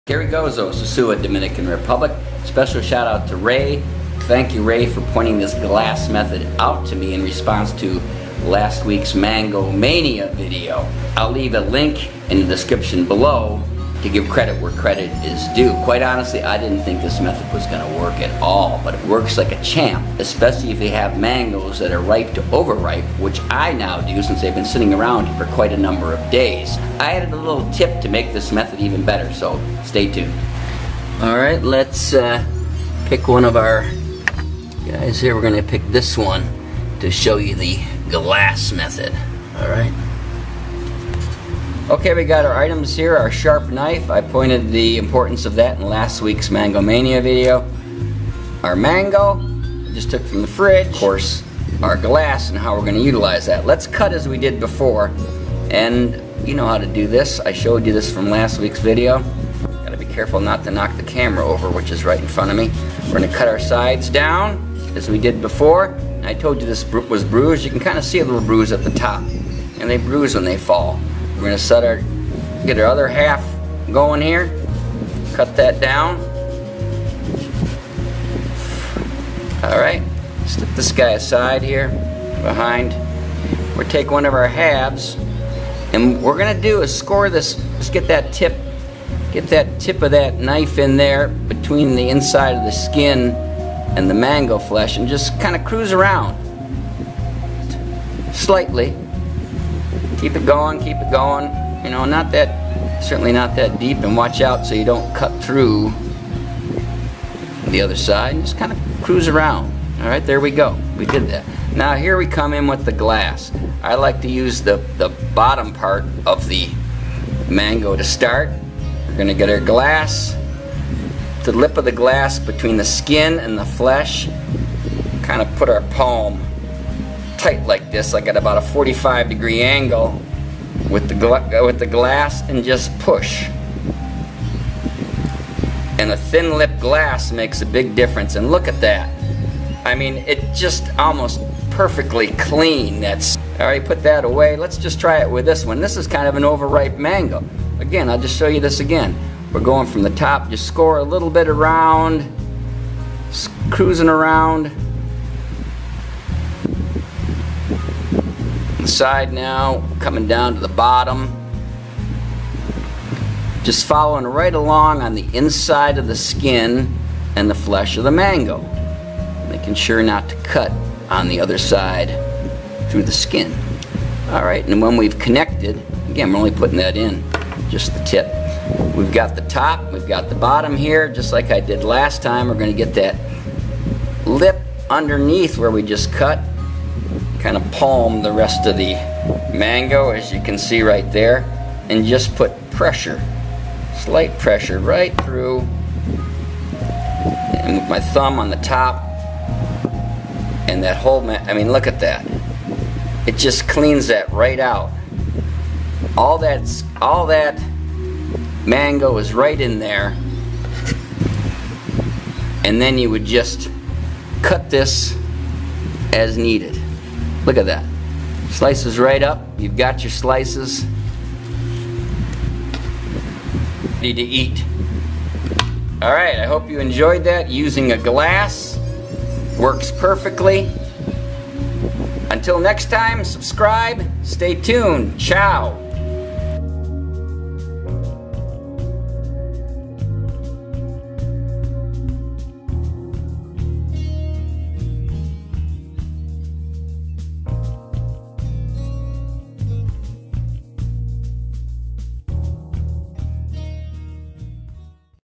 Mango Mania 2 (Peel A Mango Using A Glass, BETTER WAY), Sosua, Dominican Republic
No sooner did I post last week's video (MANGO MANIA: HOW TO SLICE AND DICE) that I heard from my buddy, Ray, who sent a video link (referenced below) and stated, 'this is how the pros do it' 😅.  After having serious doubts, I decided to try the 'gIass method' to peel a mango and, was so impressed, that I decided to create a video (MANGO MANIA 2: PEEL A MANGO USING A GLASS, BETTER WAY) and add a slight twist to make this method even better.    PRO TIPS: make a slight incision between the skin and mango all the way around before using the glass AND Use a thin-lipped glass for improved peeling.  Here's to happy mango eating and peeling.  Clearly a better way to peel a mango using a glass.

Special thanks to Ray Q. for the inspiration behind this video

From YouTube Creator Melissa Bailey 'How To Peel A Mango With A Glass'